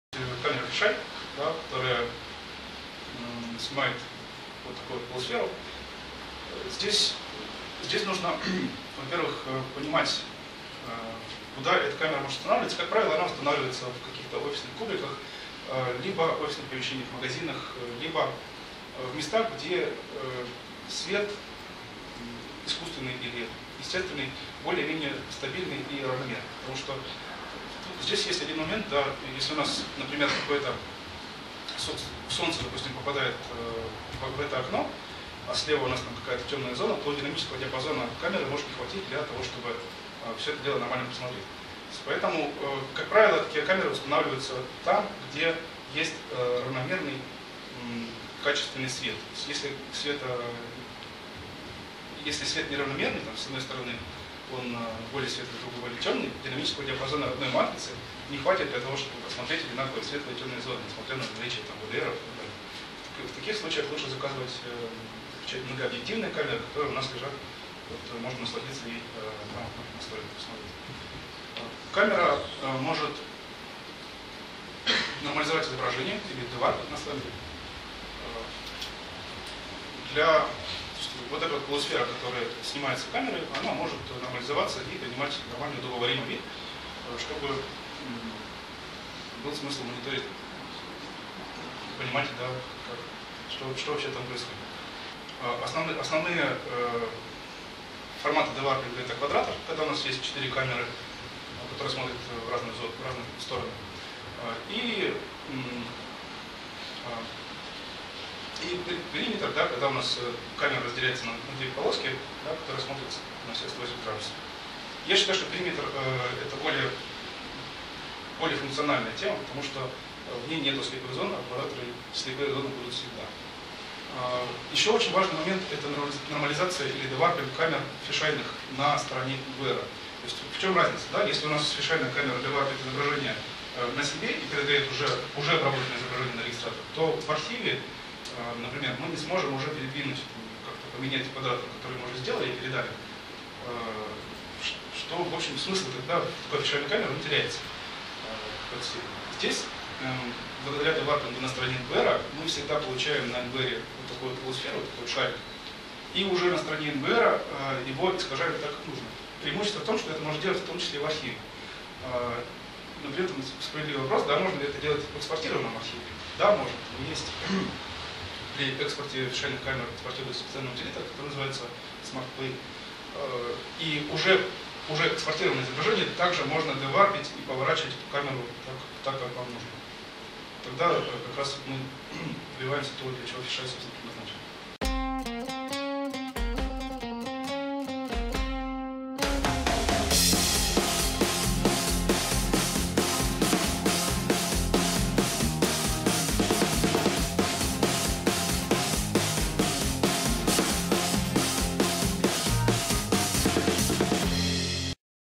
Фишай и равномерность освещения
Сергей Чуйков, "Honeywell"
Это камера фишай, которая занимает вот такую полусферу.
Здесь нужно понимать, во-первых, куда эта камера устанавливается. Как правило она устанавливается в каких-то офисных кубиках, либо в офисных помещениях, в магазинах, либо в местах, где свет искусственный или естественный более-менее стабильный и равномерный, потому что здесь есть один момент.
Если у нас, например, какой-то… солнце, допустим, попадает в это окно, а слева у нас какая-то темная зона, то динамического диапазона камеры может не хватить для того, чтобы все это дело нормально посмотреть.
Поэтому такие камеры, как правило, устанавливаются там, где есть равномерный качественный свет.
Если свет не равномерный, там с одной стороны он более светлый, с другой более темный, динамического диапазона родной матрицы не хватит для того, чтобы посмотреть одинаково в светлой и темной зоне.